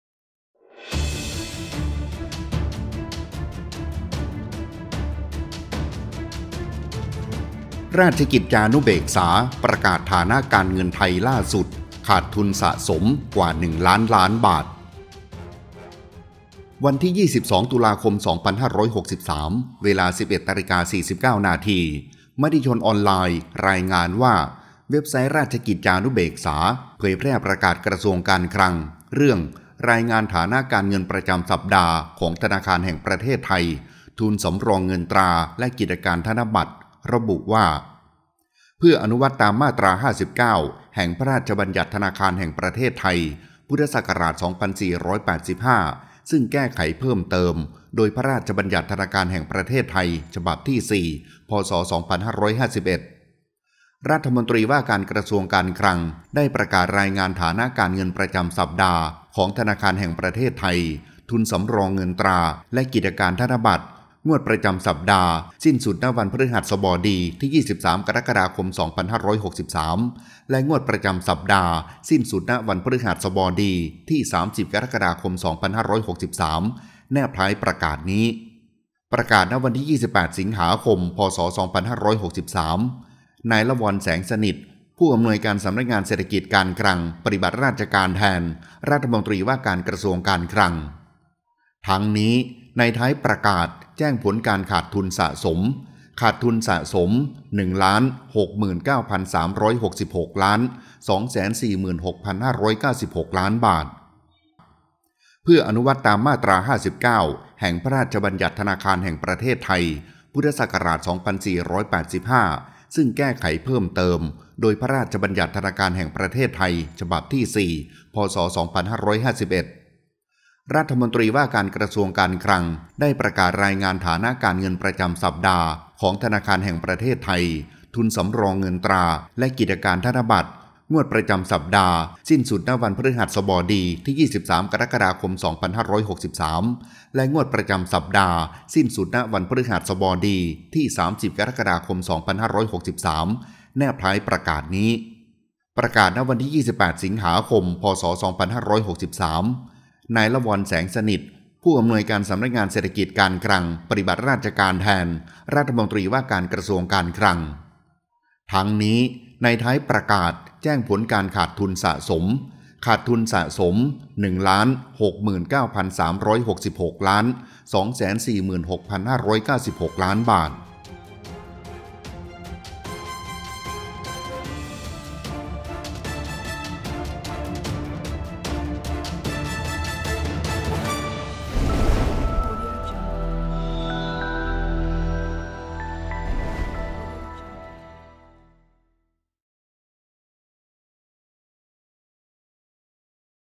📜ราชกิจจาฯ ประกาศฐานะการเงินไทยล่าสุด ขาดทุนสะสมกว่า 1 ล้านล้านบาท
ขอบคุณที่มา : matichon

.facebook.com/SP-Story-Channel-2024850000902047 เชิญติดตามข่าวสารอื่น ๆ และพูดคุยทักทายกับเราได้ครับ